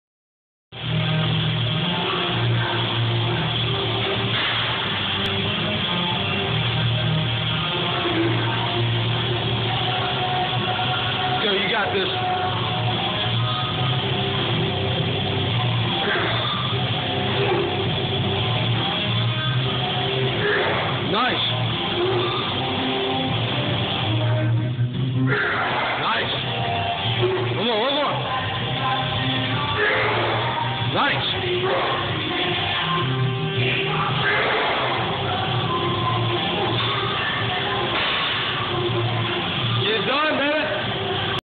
225lb overhead press
Evan reppin out 225 on shoulder press. Beast.